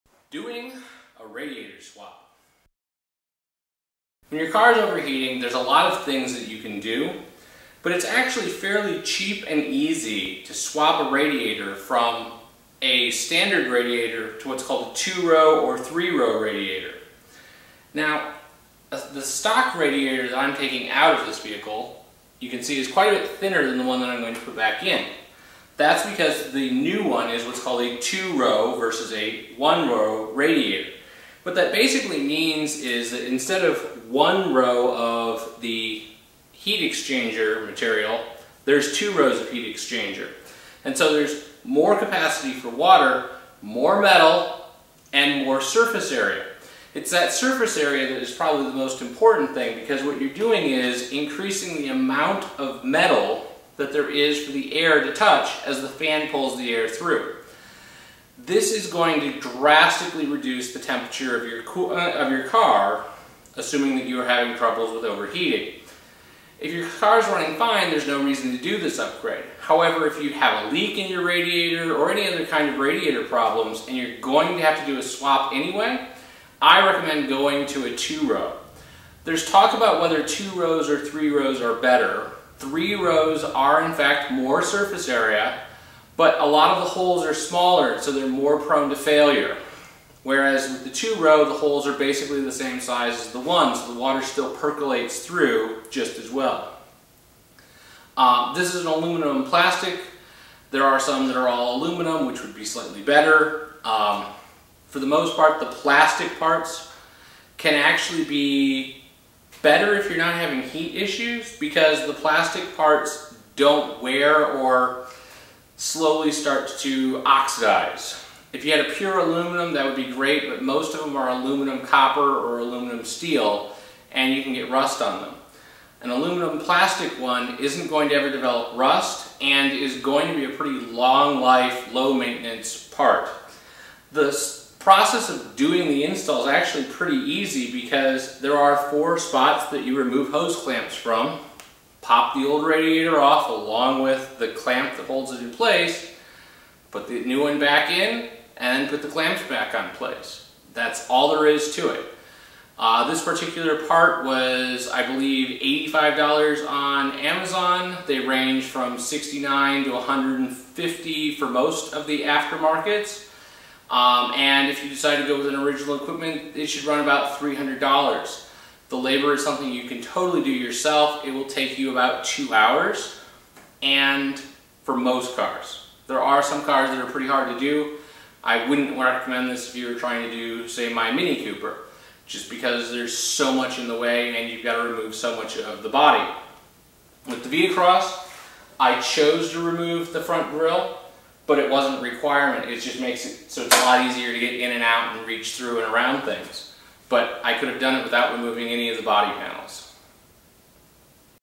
Radiator Swap: Upgrading the Radiator to a 2 Row Alumininum Core
2 Row radiators have twice the surface area of a single row. That doesn't mean twice the cooling since the second row is getting air heated by the first row, but it does still help. So does the increase in the ammount of water held. More water means that those short bursts of Air Conditioning won't put you over the top.

There are some down sides. They take longer to heat up which means in the winter you will be cold longer. They weigh more which is not ideal if you are building a race car. But for the most part two core radiators are better than their single row counterparts.

I also did my swap to performance coolant which has water wetter already in it. It also uses de-ionized water which reduces scaling and improves heat transfer. This is anti-freeze as well and gives protection to -34 which is colder than I'm likely to see, but may not be enough for all climates.

I didn't get all my hoses clamped down the first time and leaked on the floor when I got up to pressure. So don't make that mistake like I did

After making the swap make sure you check your thermostat to be certain the radiator is working as it should.